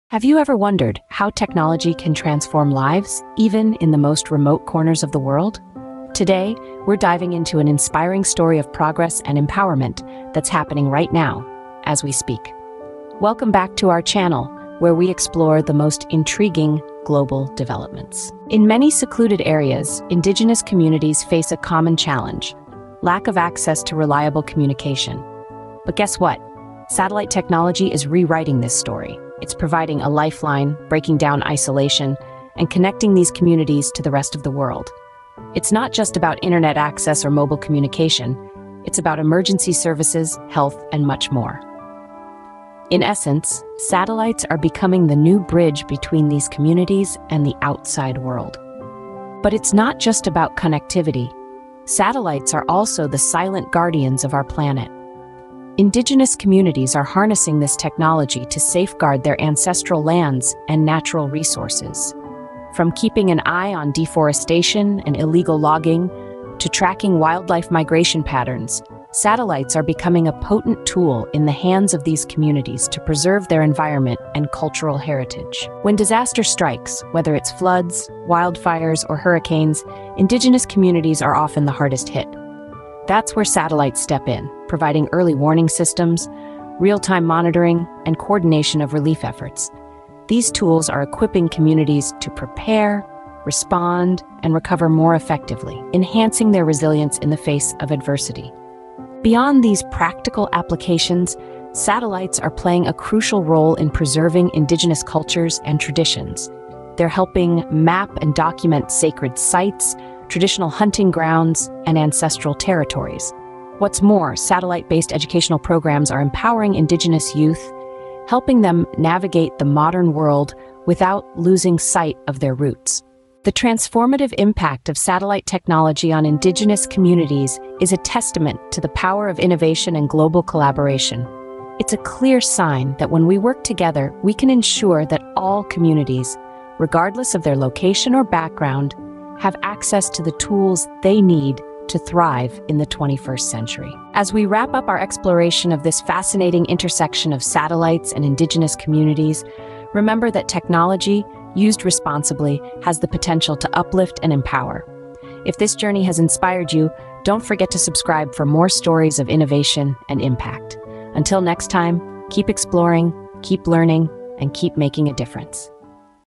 Satellites Empowering Indigenous Communities
Unlocking the Power of Satellite Technology: Empowering Indigenous Communities Worldwide

In this groundbreaking video, we delve deep into the transformative role of satellite technology in empowering indigenous communities across the globe. Join us as we explore the ways in which satellite technology has revolutionized communication, connectivity, and access to vital resources for these marginalized communities.

Discover how these innovative satellites have bridged the gap between remoteness and connectivity, providing indigenous peoples with a voice and platform to showcase their invaluable traditions, cultures, and knowledge to the world. We'll showcase real-life success stories of how satellite technology has been a catalyst for change, bringing education, healthcare, and economic opportunities to the forefront.

Through breathtaking visual examples and expert interviews, we'll take you on a journey to understand the immense impact satellite technology has had on indigenous communities, breaking down barriers and providing equal opportunities. Witness how it has empowered these communities to preserve their ancestral lands, traditions, and languages for generations to come.

By shedding light on the invaluable role of satellite technology in promoting inclusivity and sustainability, we hope to ignite a global conversation. Join us in spreading awareness by liking, sharing, and engaging with this powerful video. Together, let's celebrate the potential of satellite technology in empowering indigenous communities and promoting a more equitable world.

OUTLINE:

00:00:00 Introduction
00:00:18 Lack of Communication in Indigenous Communities
00:00:49 Satellites as Guardians of the Planet
00:01:15 Satellites in Disaster Management
00:01:39 Preserving Indigenous Cultures and Traditions
00:02:03 The Transformative Impact of Satellite Technology
00:02:24 Conclusion